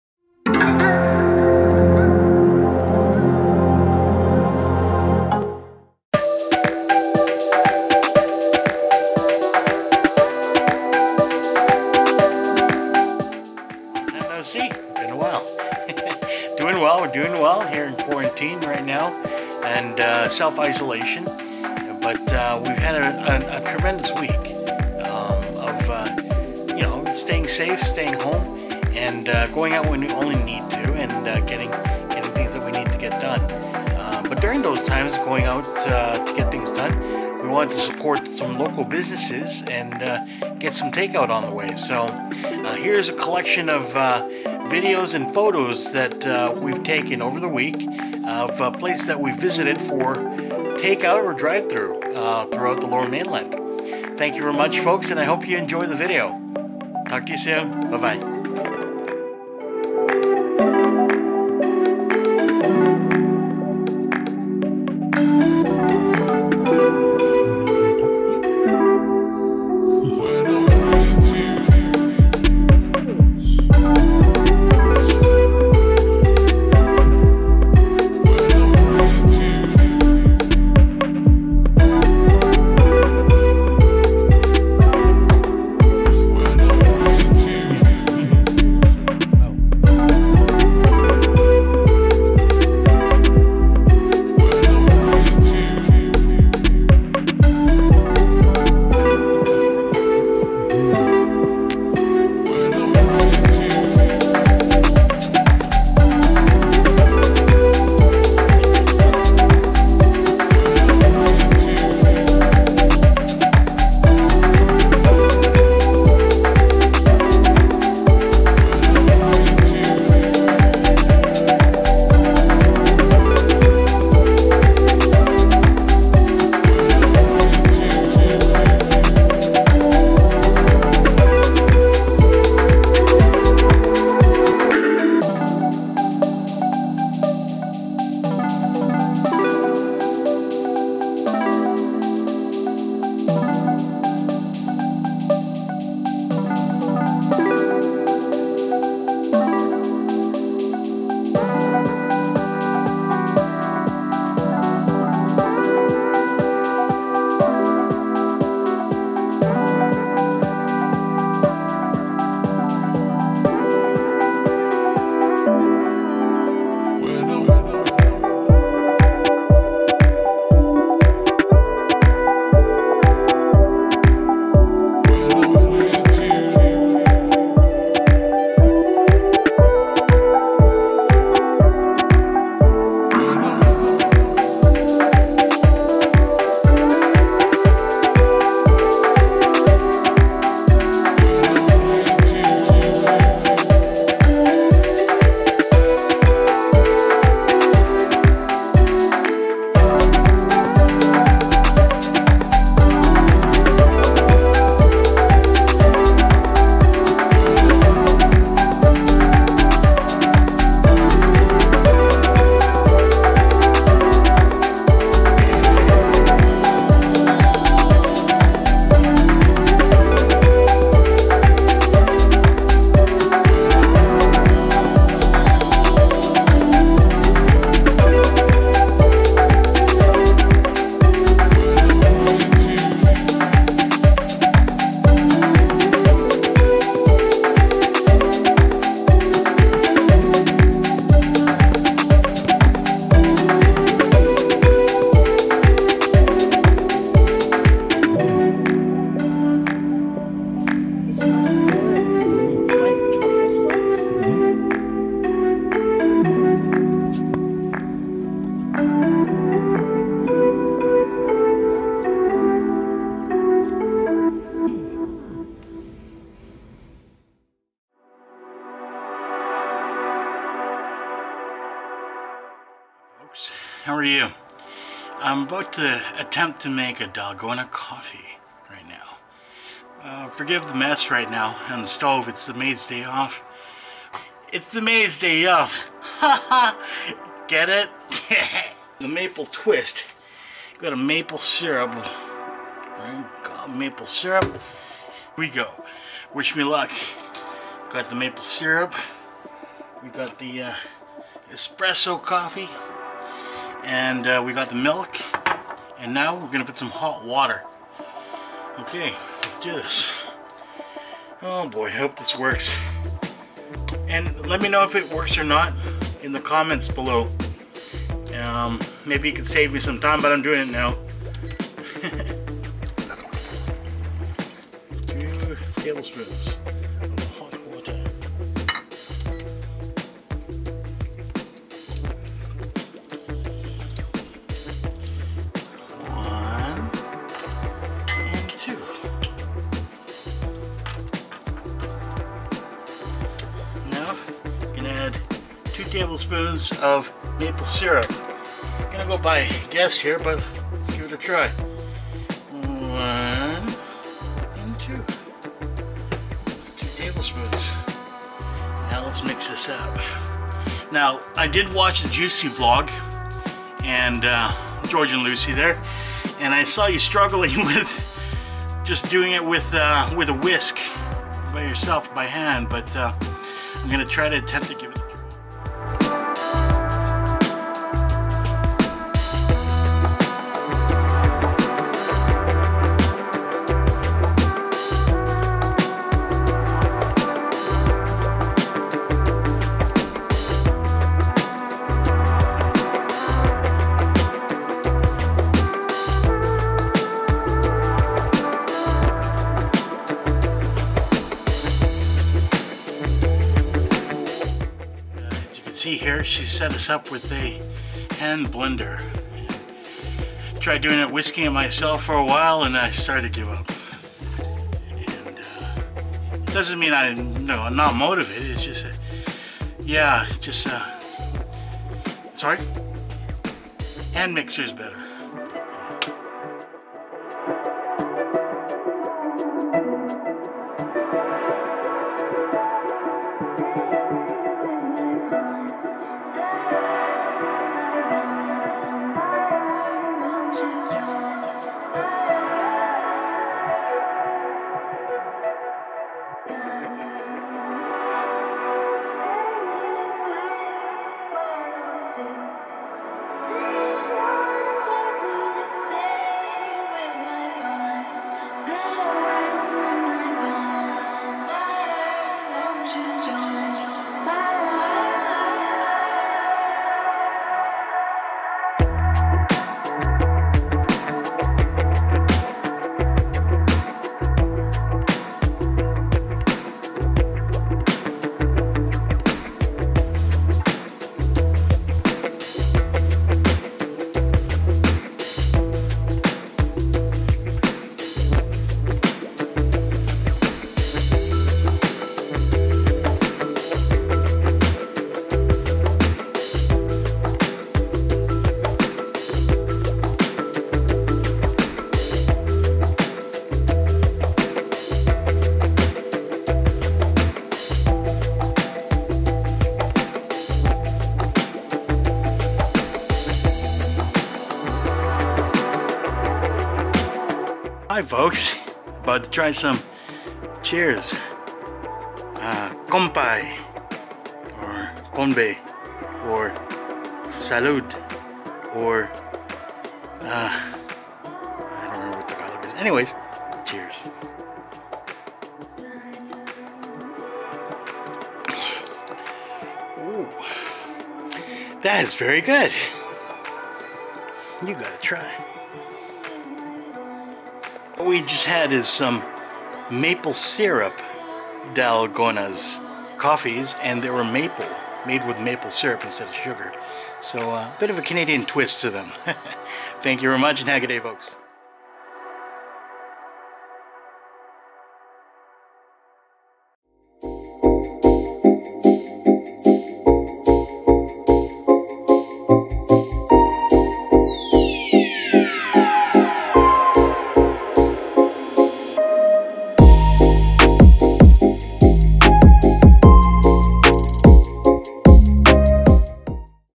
This weeks food explorations during quarantine in Metro Vancouver and DIY Maple Syrup Dalgona Coffee
A good quarantine week to you! Baby and I decided to showcase some of food explorations this past week in Metro Vancouver. Since dine-in is temporarily suspended at restaurants in Vancouver, Baby and I have relied on takeout and delivery for our dining options this week. We finished off with attempting to create Maple Syrup Dalgona Coffee for our first time this evening. Enjoy! VLOG 134